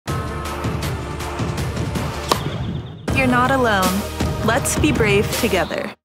Maybelline New York podržava UNICEF Brave Together
Kada su prijatelji u teškoj situaciji, možeš biti prijatelj koji zna kako da ih podrži. Ohrabri ih da potraže stručnu pomoć.
Maybelline New York podržava UNICEF u borbi za podizanje svesti o mentalnom zdravlju mladih. Podeli svoj problem kroz chat na sajtu svejeok.rs svakog dana od 18h do 22h.
Nisi sam/sama. 💗